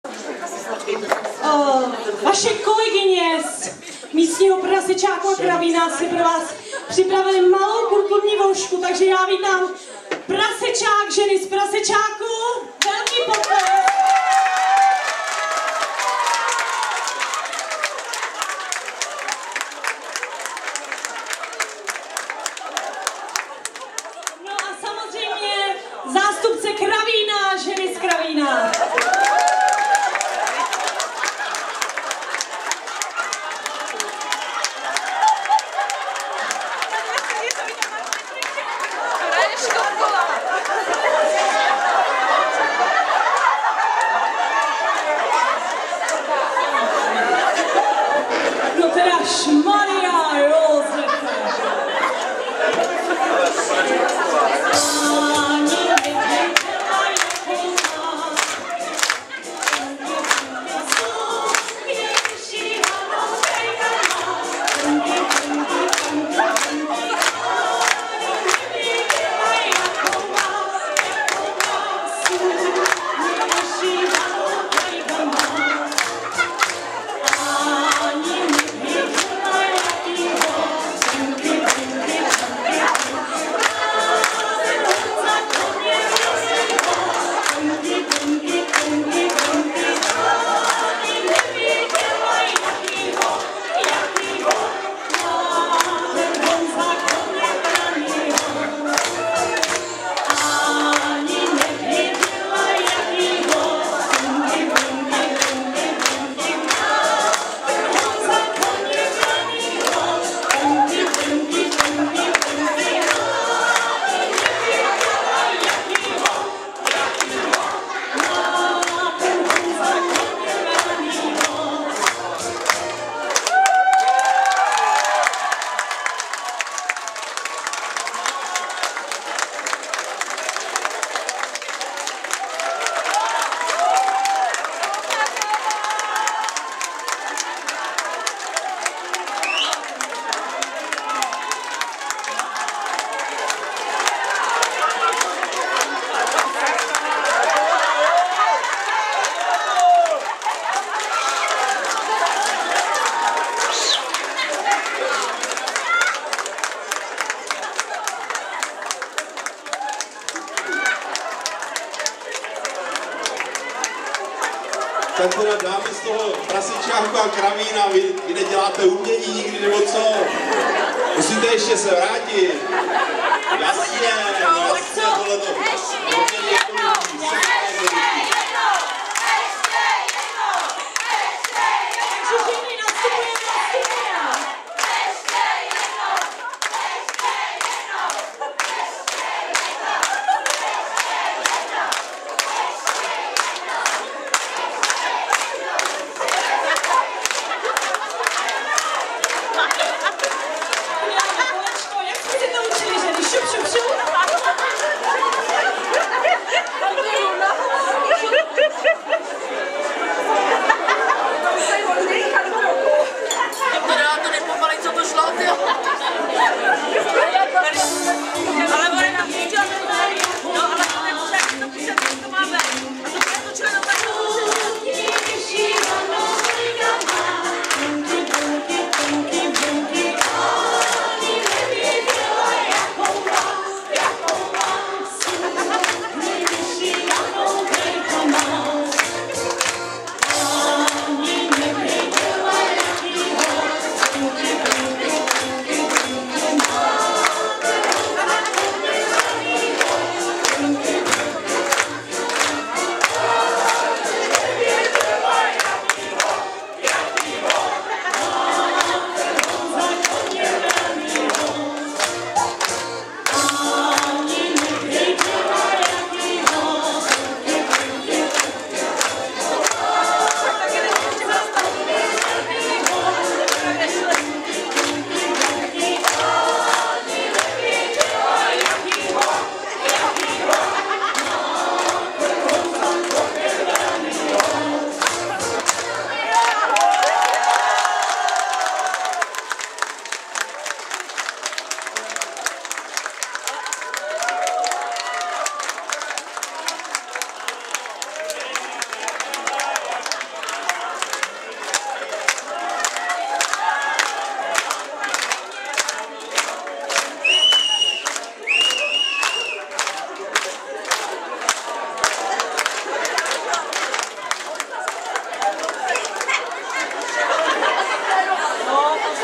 MDŽ 2010 Vepříkov 3
Malé překvapení pro ženy připravili dobrovolní hasiči (ženy z místního kravína a prasečáku :-))))  )